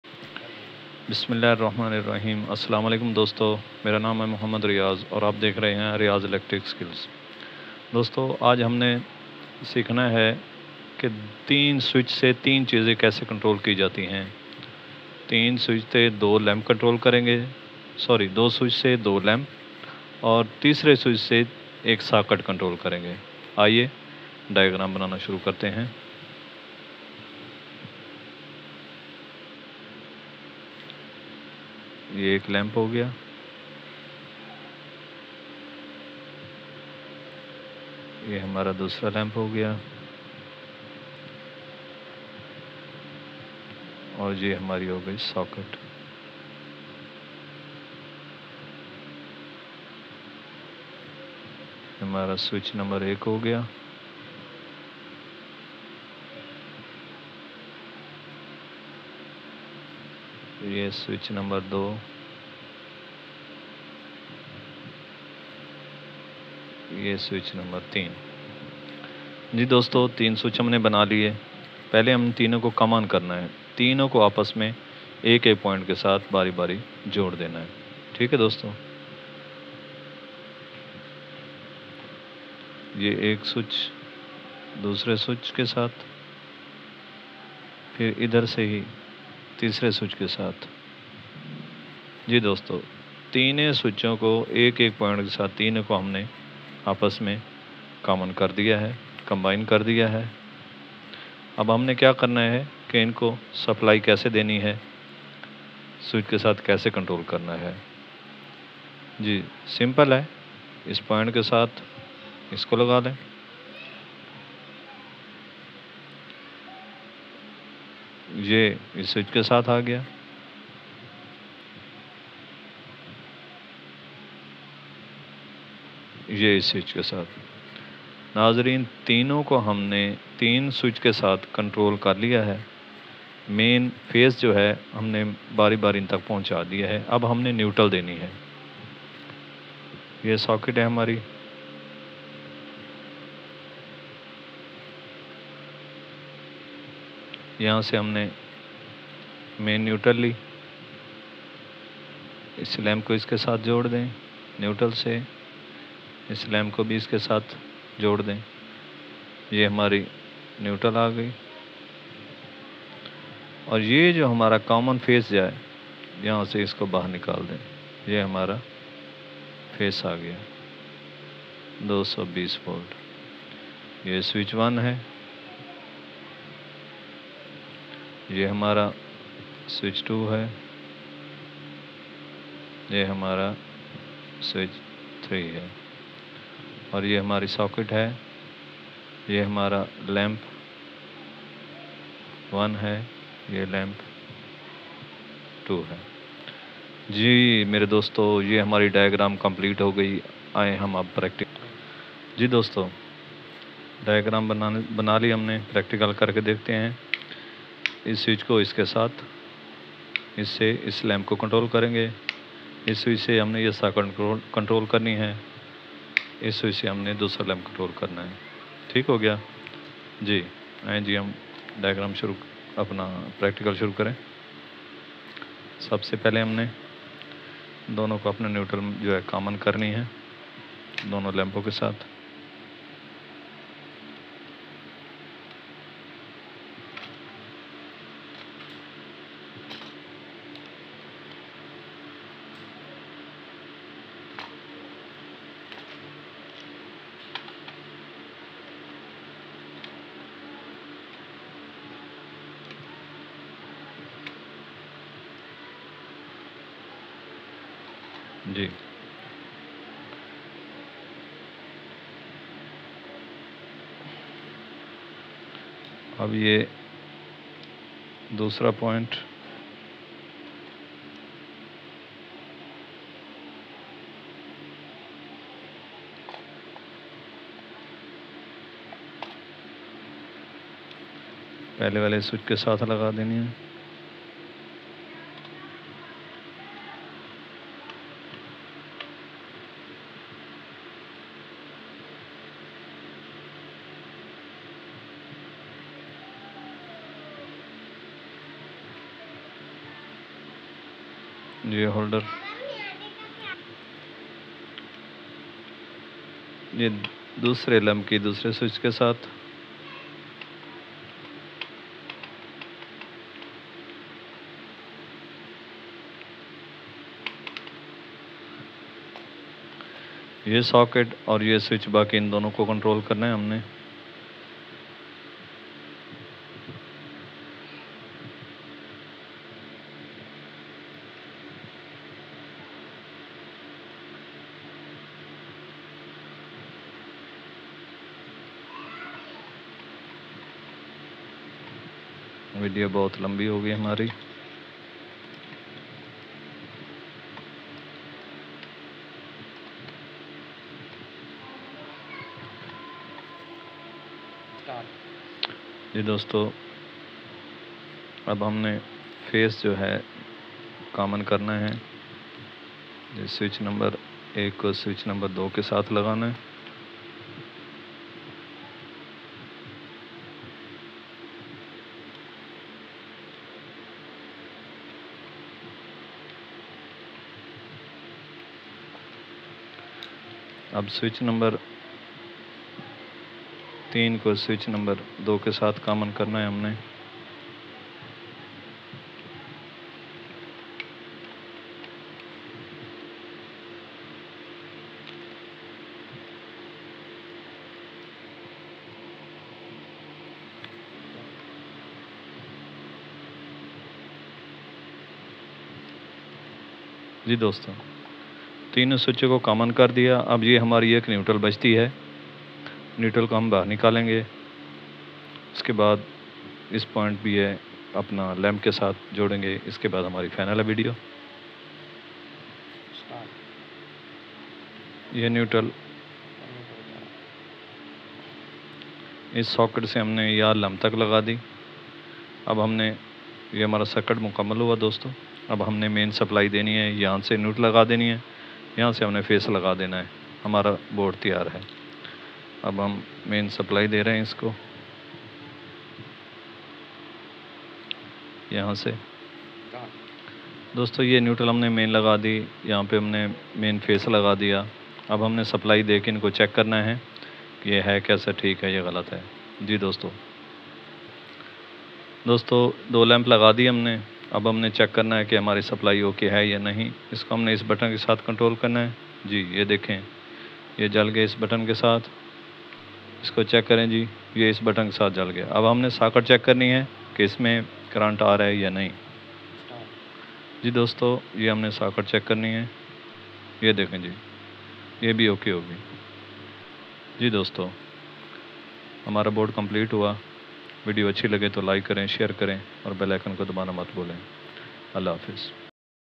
How to Control Two Lamps and One Socket with Three Switches
How to Control Two Lamps and One Socket with Three Switches | Ultimate Guide for Home Automation | RiazelectricSkills

Video Description:
In this comprehensive guide, learn the art of controlling two lamps and one socket using just three switches. Dive into the world of home automation and discover the power of efficient lighting control. Whether you're a DIY enthusiast or a tech-savvy homeowner, this step-by-step tutorial will equip you with the knowledge and skills to streamline your lighting setup. Say goodbye to tangled cords and hello to convenience and efficiency!